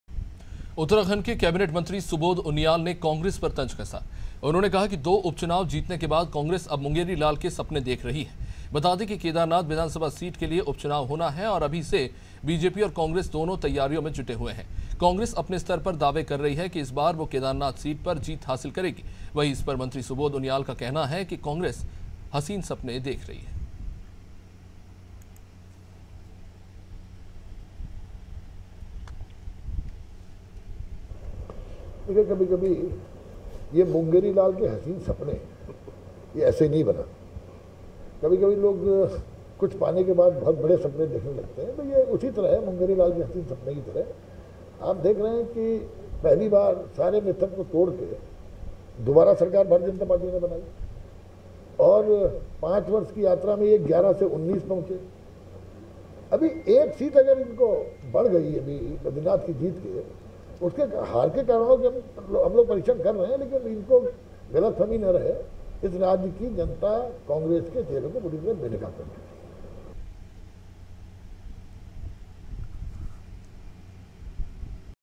Dehradun : केदारनाथ विधानसभा सीट पर उपचुनाव, मंत्री सुबोध उनियाल का कांग्रेस पर तंज || Anaadi Tv
AIRTEL - 379